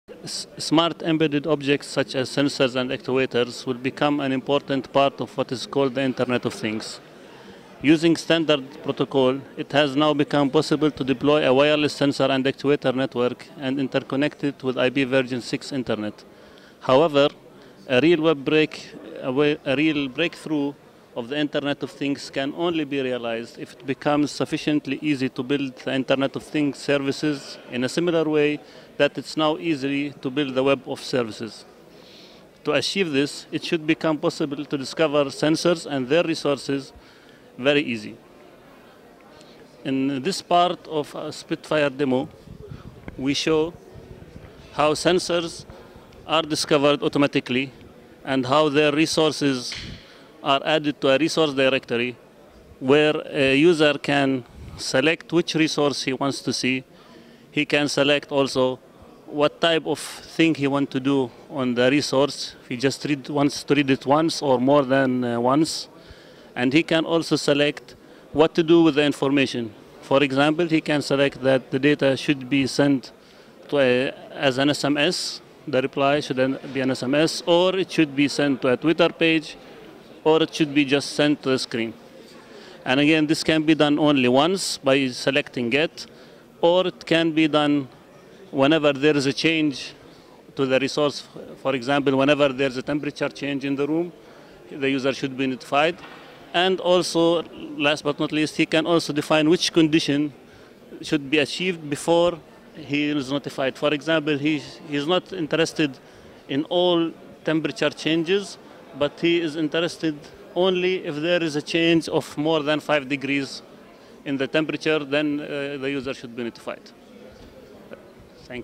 SPITFIRE Demo 2: True Self-Configuration for the IoT - Hands-On FIRE! 8-10 May 2013 / FIA-Dublin
For the Internet of Things to finally become a reality, obstacles on different levels need to be overcome. This is especially true for the upcoming challenge of leaving the domain of technical experts and scientists. Devices need to connect to the Internet and be able to offer services. They have to announce and describe these services in machine-understandable ways so that user-facing systems are able to find and utilize them. They have to learn about their physical surroundings, so that they can serve sensing or acting purposes without explicit configuration or programming. Finally, it must be possible to include IoT devices in complex systems that combine local and remote data, from different sources, in novel and surprising ways.
This demo shows how all of that is possible today. Our solution uses open standards and state-of-the art protocols to achieve this. It is based on 6LowPAN and CoAP for the communications part, semantic web technologies (RDF) for meaningful data exchange, autonomous sensor correlation to learn about the environment, and software built around the Linked Data principles to be open for novel and unforeseen applications.
Our demo application assumes and visualizes a simple building that basically includes two rooms, i.e, bedroom and living room. The visitor will be able to interact with the building by affecting basic environmental conditions, such as the lightning, the presence of people, etc. The building will include off-the-shelf wireless sensor and actuator devices to demonstrate the usage of the above mentioned technologies. Furthermore the visitor will be able to add new wireless devices to the building and observe the automatic integration of the new devices in a plug-and-play manner (but without the need to plug anything).
We demonstrate an easy-to-use GUI to find and utilize devices as mentioned in the beginning. It is based on HTTP and by this means usable from anywhere in the world. This GUI enables the visitor to define rules to control the buildings behavior, e.g. the air conditioning, by using both actual measurements from surrounding sensors and data from the LOD cloud.